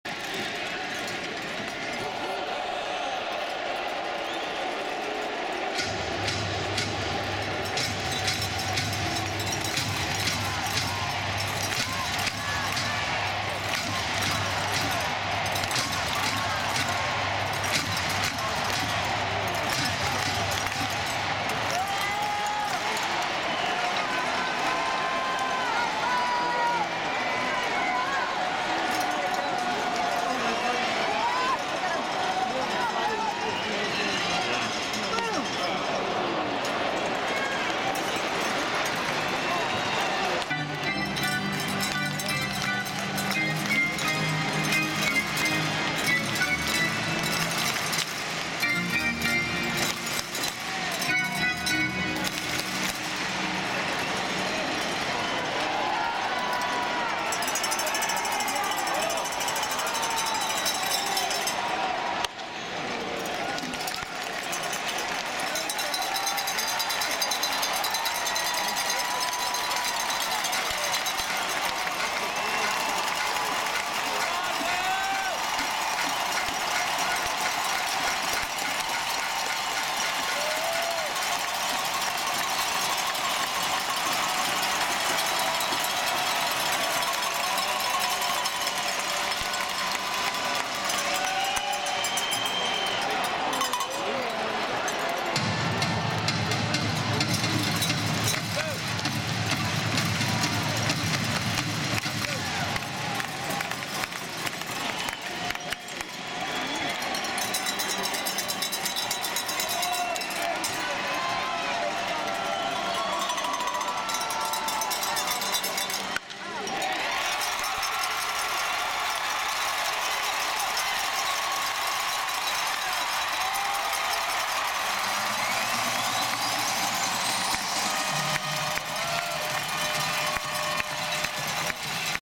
5-11-14 Will Myers Singles home Evan Longoria against Cleveland
Cleveland Indians at Tampa Bay Rays:
W Myers singled to right, E Longoria scored, J Loney to second.